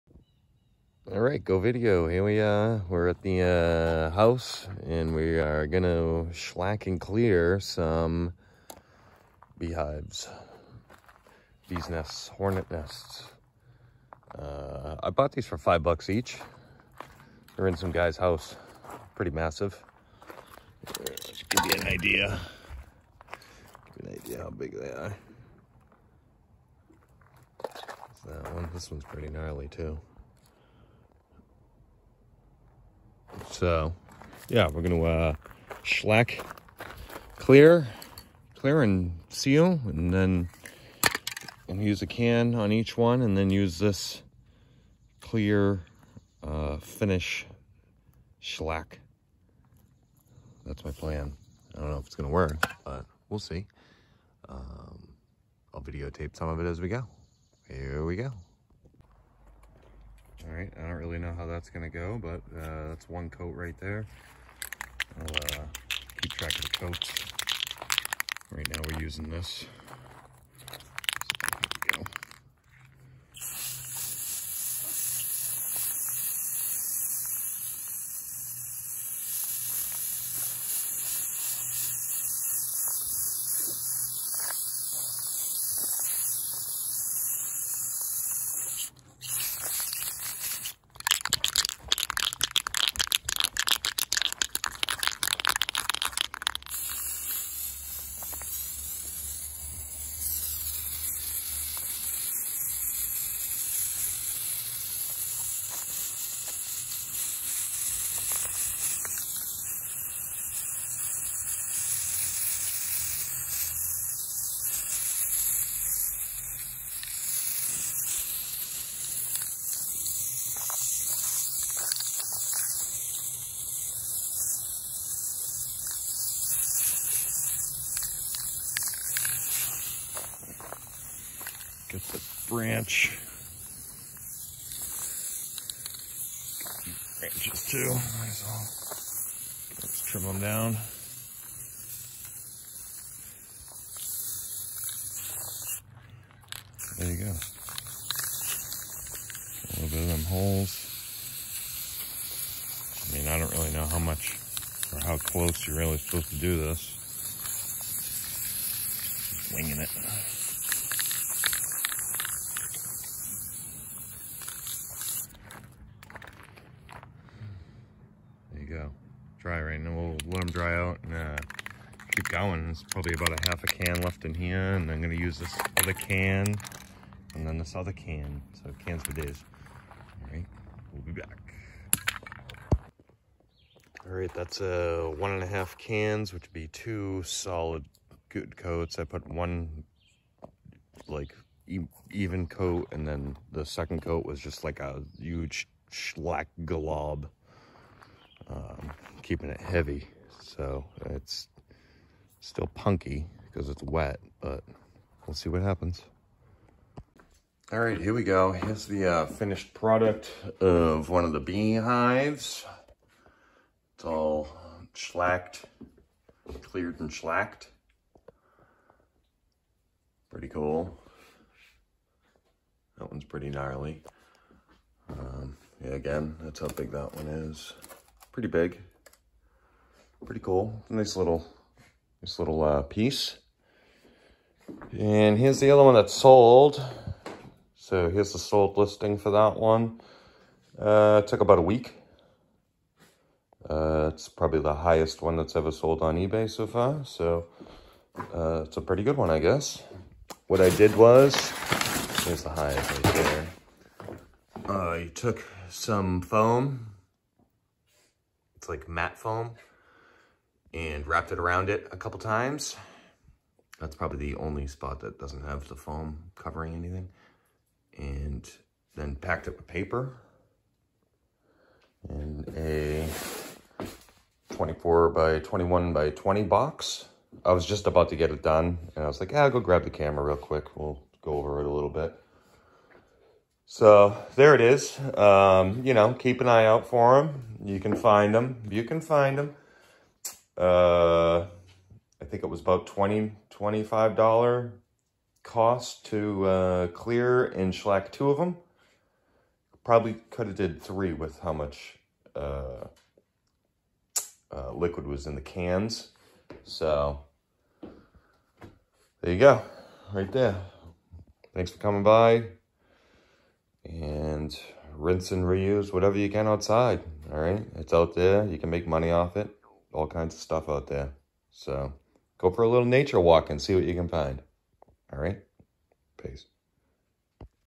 BEE BOLO - Beecause I wanted To - Beecause I Can
Selling a wasp nest on ebay
Highest sold in 90 days
I set the bar high Bahhhhhhh